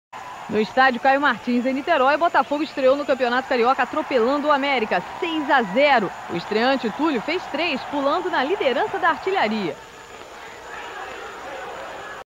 Botafogo 6x0 América - Campeonato Carioca 1994 - Turno
Campeonato Carioca 1994 - I TURNO

30/Jan

Botafogo 6-0 América

Local: Caio Martins, Niterói

Juiz: Jorge Emiliano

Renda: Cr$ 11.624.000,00

Público: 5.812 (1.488 não pagaram ingressos)

Gols: Regilson 4’, Roberto Cavalo 12’, Túlio 49’, 52’ e 88’, Marcelo 71’

Cartão amarelo: Regilson e Rogério

Botafogo: Vagner, Eliomar, Márcio, André e André Duarte; Nélson, Roberto Cavalo (Bobi), Dedé e Regilson; Róbson (Marcelo) e Túlio. Técnico: Dé

América: Vitor, Leonardo, Tino, Marcelo Vaz e Vanderlan; Rogério (Carlinhos), Luisinho, Brandão (Gilberto) e Biro; Sandro e Joélton. Técnico: Washington Ribeiro.